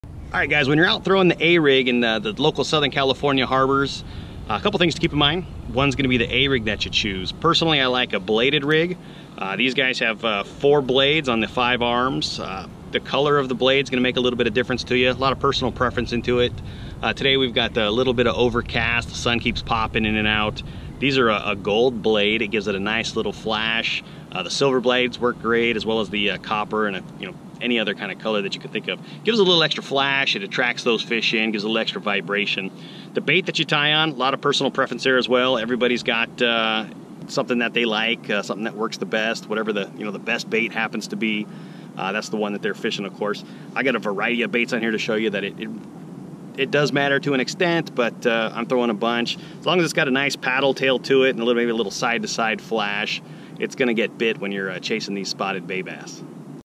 Tips on Selecting and Fishing the A-Rig
In this YouTube Short, I give a few tips on selecting and fishing the A-Rig for spotted bay bass, calico bass and sand bass.
This is an incredibly effective technique to chase these saltwater bass around and I hope this gets you dialed in just a bit.